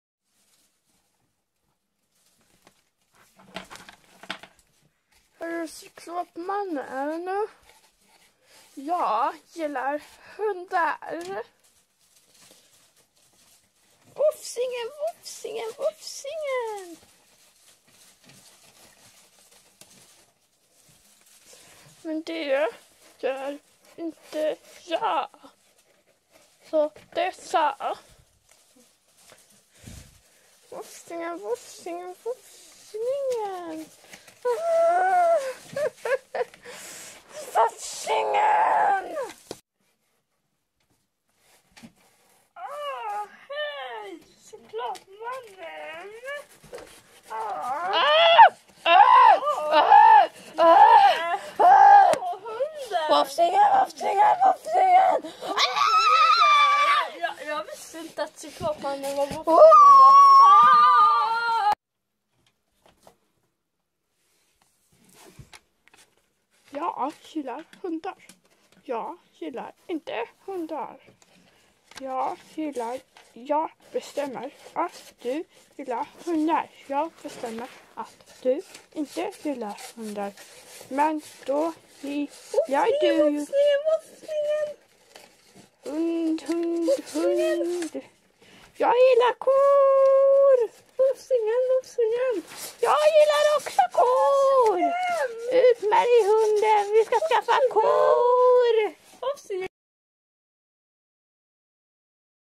teatern ep 1 2 3
Jag lägger ihop ep 1 2 och 3 av teatern i en längre video